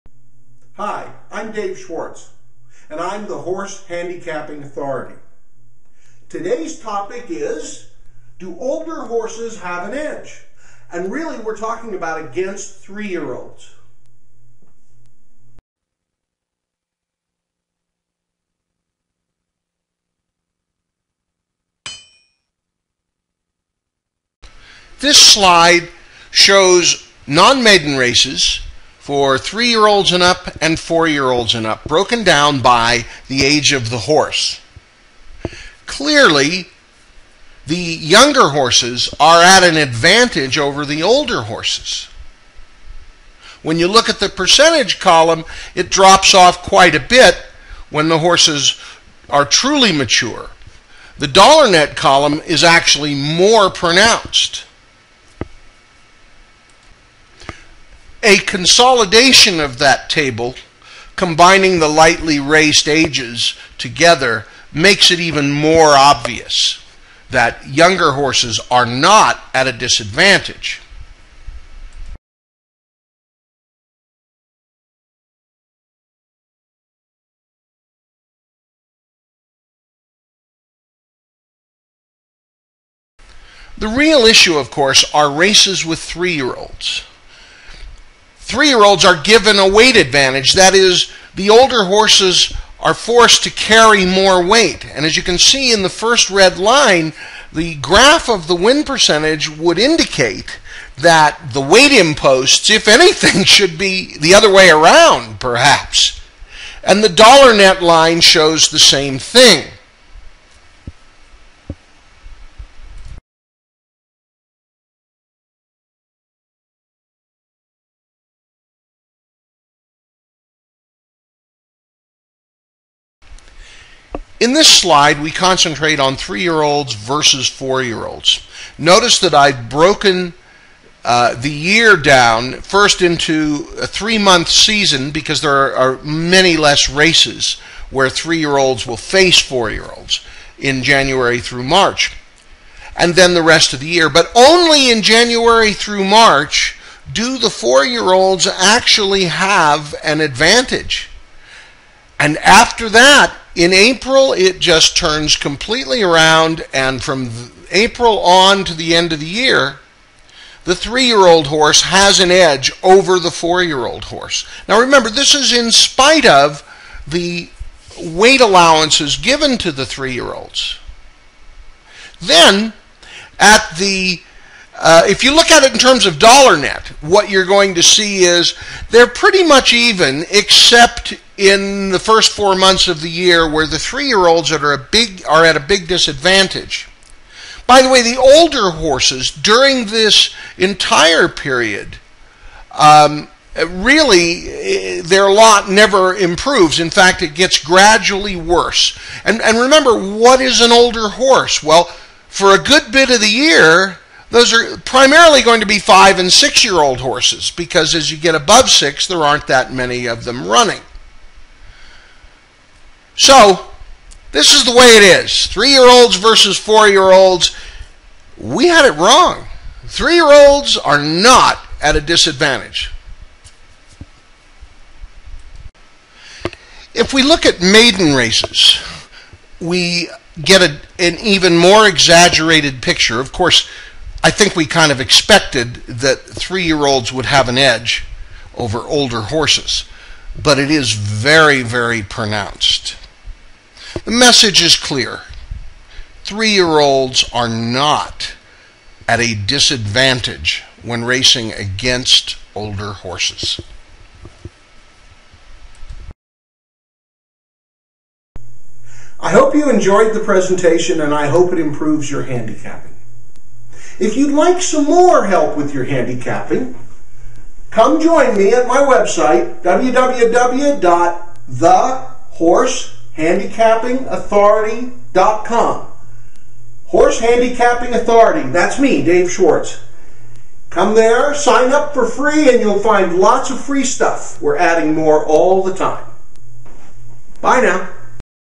Improving Your Game: Do Older Horses Have an Edge | Episode 2
Horse Handicapping Authority Dave Schwartz gives you tips in: Improving Your Game | Episode 2.  Do older horses have an edge against 3 year olds?  The results may surprise you!